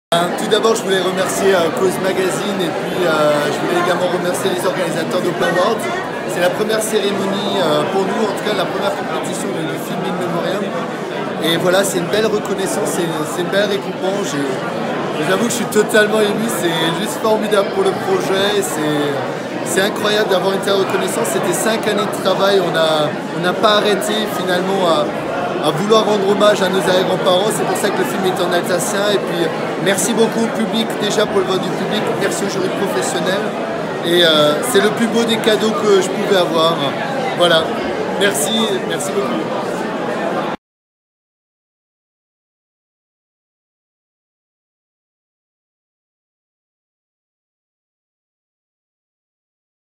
Le réalisateur Benjamin Steinmann Lauréat 2 Awards - Interview Hopl'Awards
In Memoriam
Un film en hommage aux Malgré-Nous
Écrit et réalisé par Benjamin Steinmann (2019)

Cérémonie Hopl'Awards 2019
Award Production Audiovisuelle Alsacienne
Award Prix du Public

Synopsis Film In Memoriam |
Août 1942, les Alsaciens et les Mosellans sont incorporés de force dans l’armée allemande durant la seconde guerre mondiale. Certains sont incorporés dans les unités de la Wehrmacht, la Kriegsmarine (marine allemande), la Waffen SS dites Unité d’élites, plusieurs se retrouvèrent principalement sur les fronts russes à combattre l’armée de Joseph Staline. André Balzinger, jeune incorporé alsacien, décide de déserter les fronts russes pour rejoindre la Résistance ou la France libre. Accompagné par d’autres soldats alsaciens, ils partent sur le chemin du retour, sans identité, sans patrie.Traité de lâches par les allemands et d’ennemis par les soviétiques, ces soldats sont condamnés à avancer dans l’incertitude et le doute de perdre la vie à chaque instants.